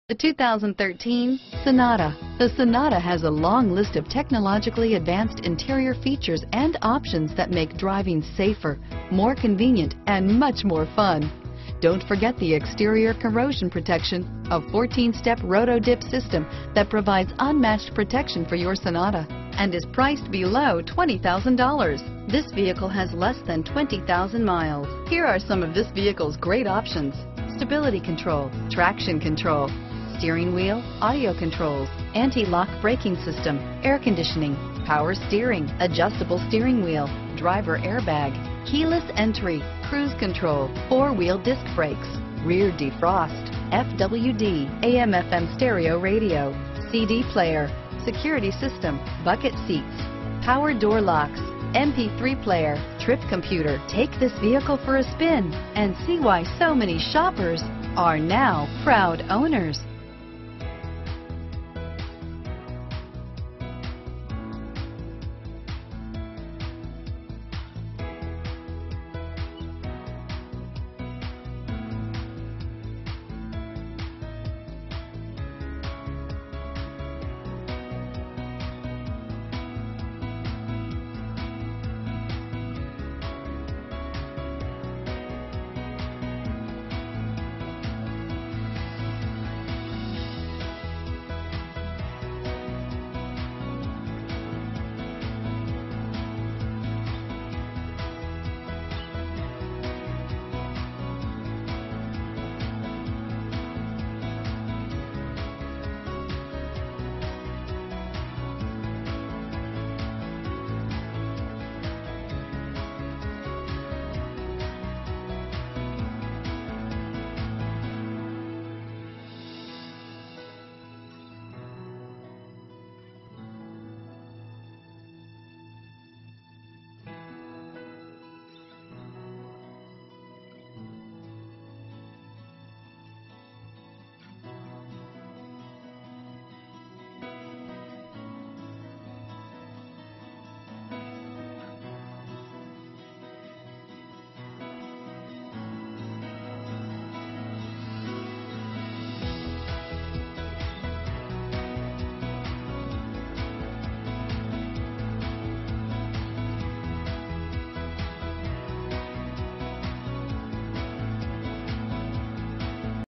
2013 Hyundai Sonata Kennesaw, Canton, Woodstock, Marietta, Alpharetta GA H6366A
Cherokee Hyundai: Servicing Kennesaw, GA near Kennesaw, Canton, Woodstock, Marietta, Alpharetta GA
2013 Hyundai Sonata GLS - Stock#: H6366A - VIN#: 5NPEB4AC6DH709797

Cherokee Hyundai
2255 Barrett Lakes Blvd

Passionate enthusiasts wanted for this dominant and dynamic 2013 Hyundai Sonata . Enjoy buttery smooth shifting from the Automatic transmission paired with this high performance Gas I4 2.4L/144 engine. With an incredible amount of torque, this vehicle needs a serious driver! It's outfitted with the following options: Pwr door locks, Cruise control, Rear window defroster, Front wheel drive, 110-amp alternator, Front/rear crumple zones, Daytime running lamps, Front/rear stabilizer bars, Occupant classification system (OCS), Traction control system (TCS) w/electronic stability control (ESC). Cruise for miles in this lively and fun-loving Sonata.Stop by Cherokee Hyundai located at 2255 barett lakes blvd, Kennesaw, GA 30144 for a quick visit and a hassle-free deal!